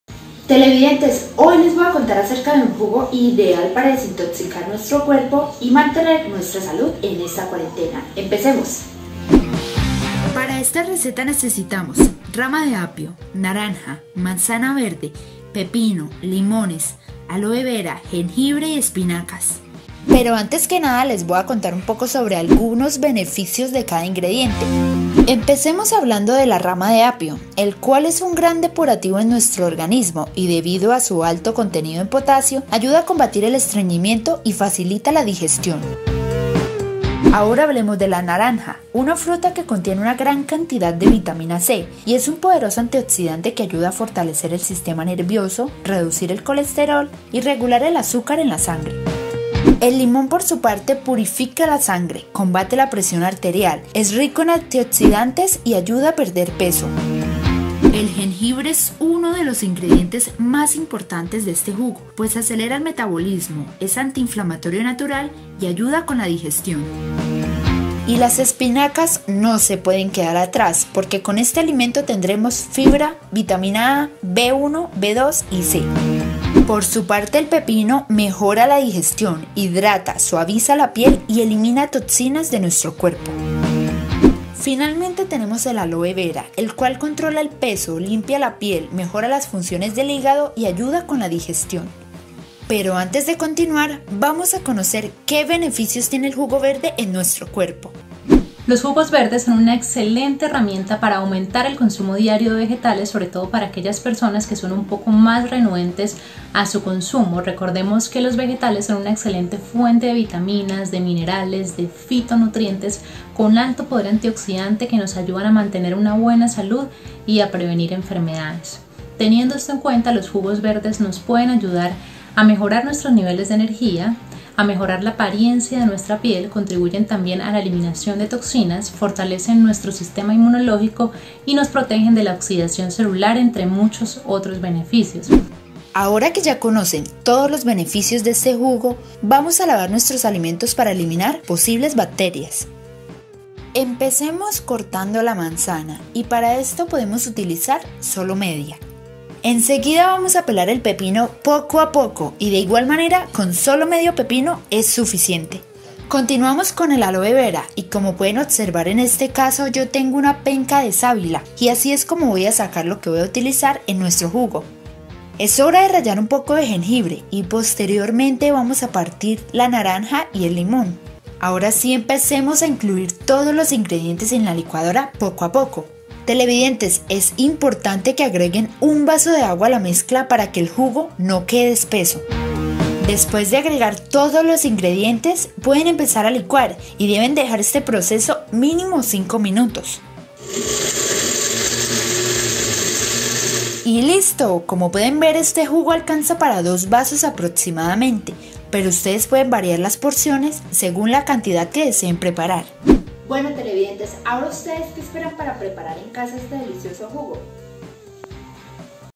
¿Conoces sobre los beneficios del batido verde?
Hoy te enseñaremos a realizar un delicioso batido verde, una receta ideal para adquirir vitaminas y antioxidantes para tu cuerpo